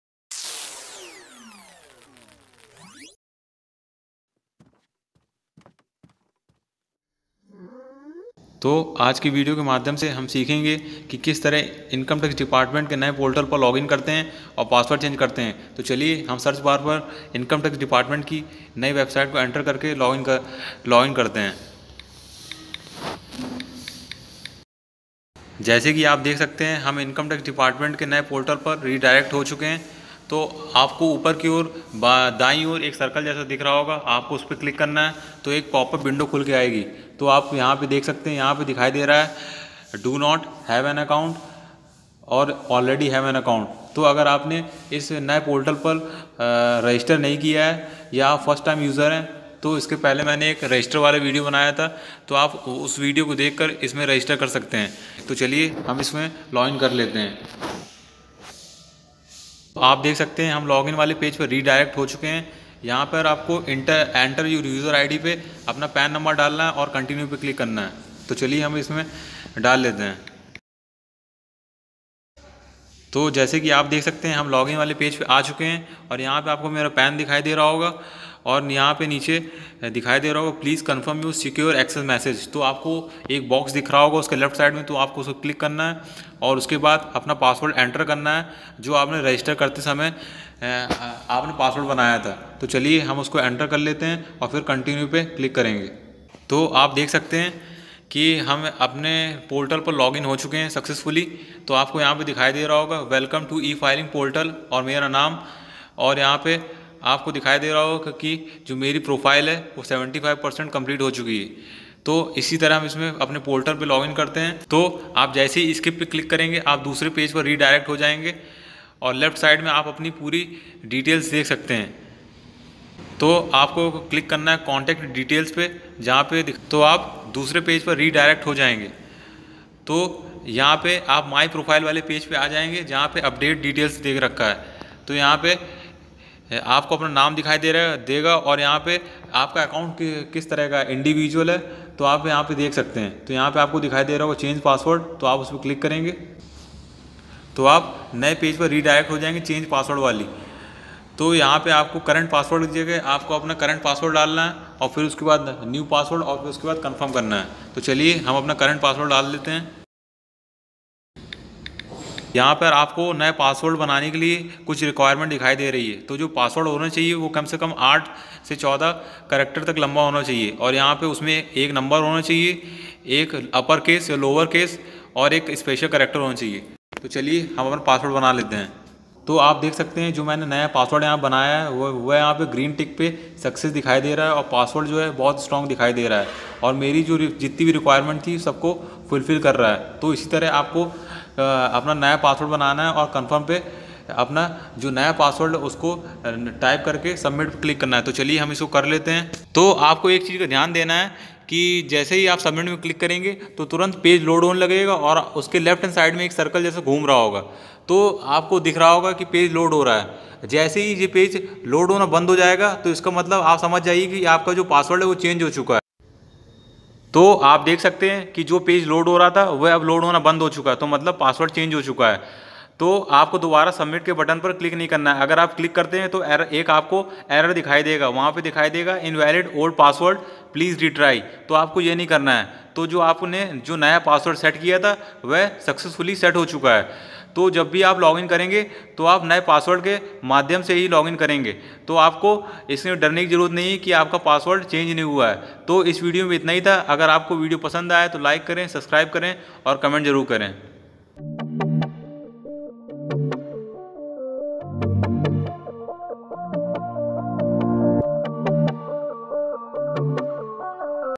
How To Login & Change Password in New Income Tax Portal 2.0 | Login और Password Change करना सीखें
नए INCOME TAX PORTAL 2.O में Login और Password बदलना  सीखें हिंदी में